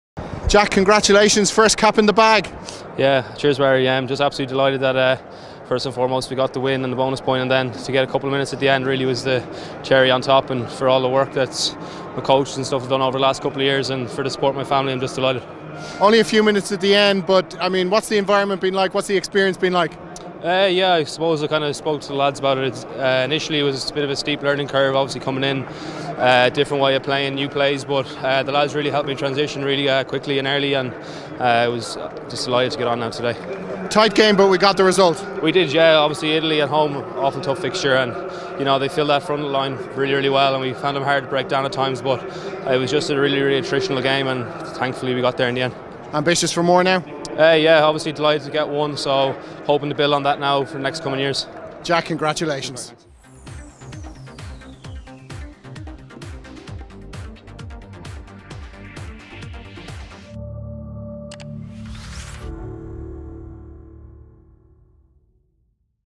Jack Carty On His First Cap
Post Match interview with Ireland's new cap Jack Carty who was delighted to make his first appearance for Ireland today.